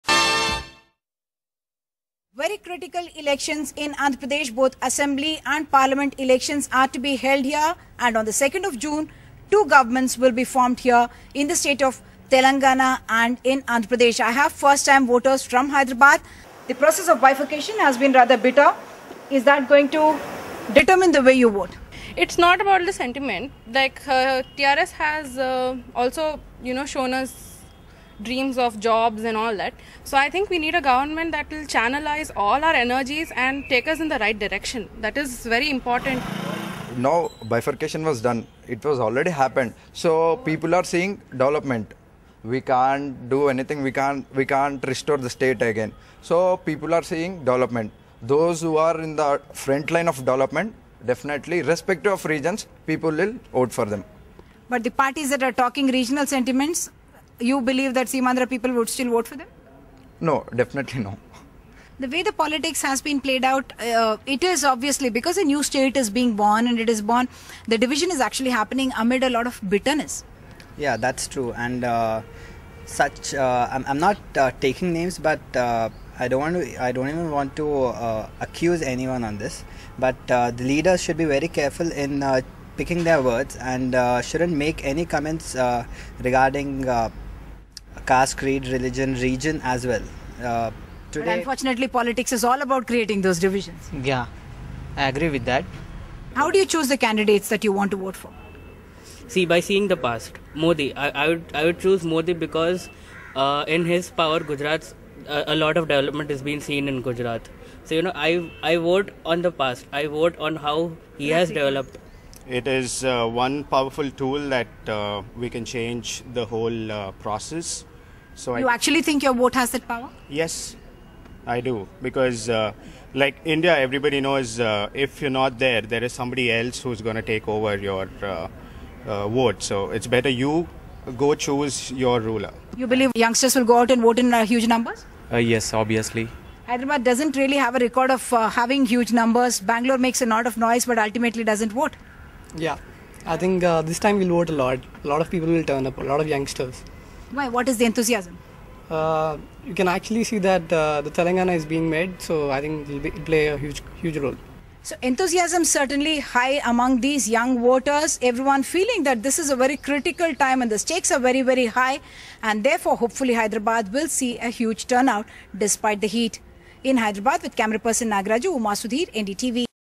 Youngsters want to fly high in Hyderabad
First-time voters in Hyderabad are anxious about the road ahead. Simultaneous elections to the Lok Sabha and the assembly in the yet-to-be-born state of Telangana took place on Wednesday. Uma Sudhir chatted with a group of youngsters about their concerns and priorities.